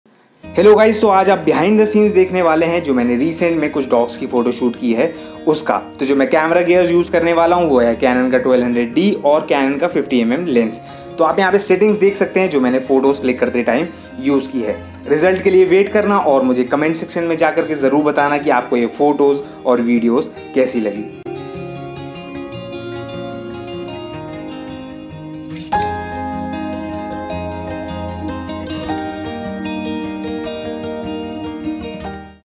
DOG PHOTOGRAPHY | BEHIND THE SCENES | CANON 1200D
hi guys, on DOG PHOTOGRAPHY. in this video you'll see some BEHIND THE SCENES of the DOG photoshoot i did recently.
camera gears used canon 1200d aand 50mm